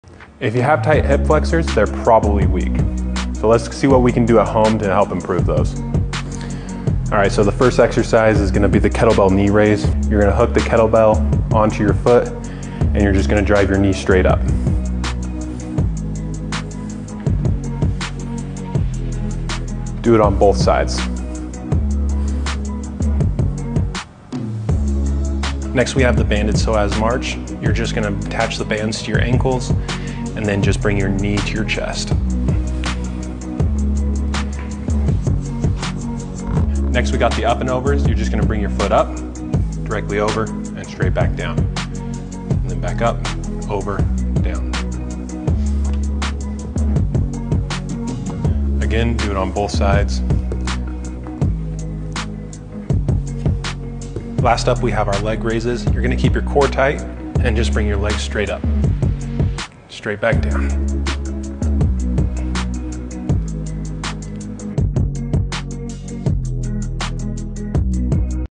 Strengthen Your Hip Flexors At Home
Have tight hip flexors? You can strengthen them at home to help. Checkout these movements and implement them in your next attempt home workout to help with the tightness.

Checkout Jayflexfitness.com for the best at home fitness gear.